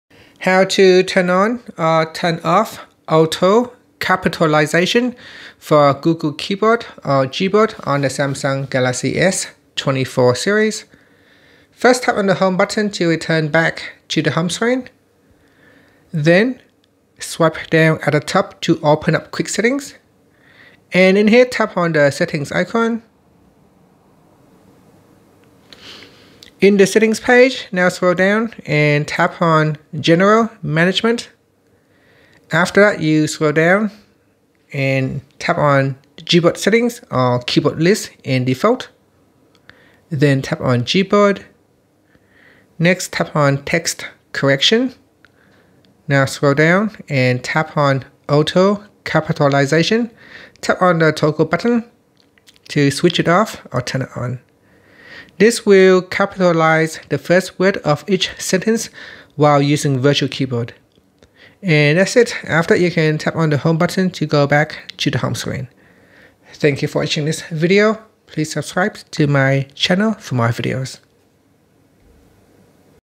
Galaxy S24/S24+/Ultra: How to Turn On/Off Auto-Capitalization For Google Keyboard/Gboard
🚀 In this tutorial, we'll guide you through the process of turning on/off auto-capitalization for Google Keyboard (Gboard) on the Samsung Galaxy S24, S24+, and Ultra. Whether you're a power user or just getting started with your new device, mastering these keyboard settings can greatly enhance your typing experience.